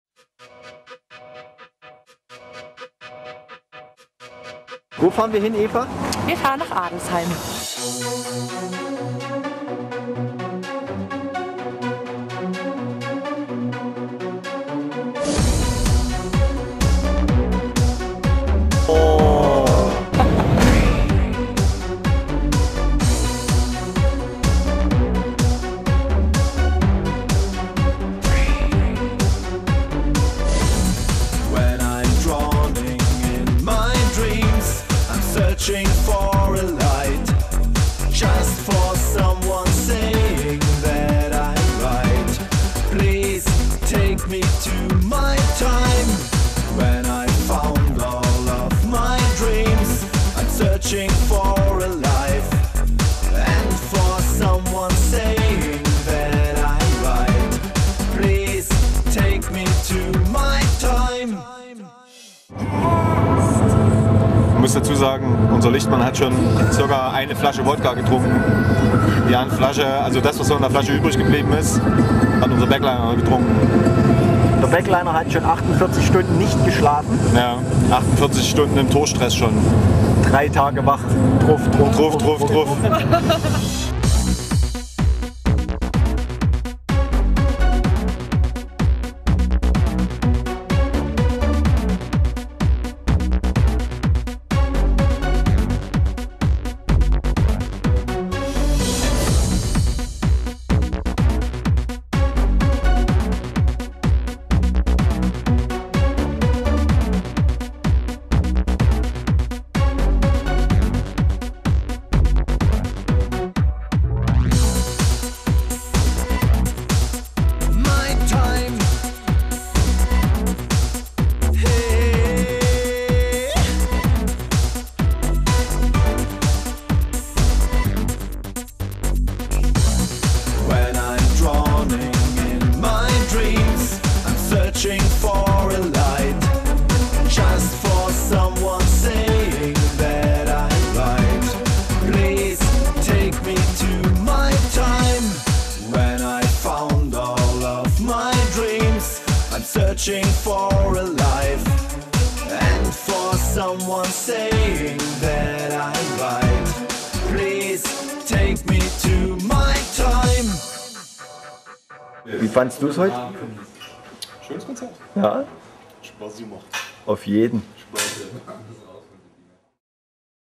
f.o.d. tour blog - adelsheim - masters of dark entertainment
f.o.d. - m.o.d.e. festival 2008 mit eisbrecher, down below, fabrik c u.v.m.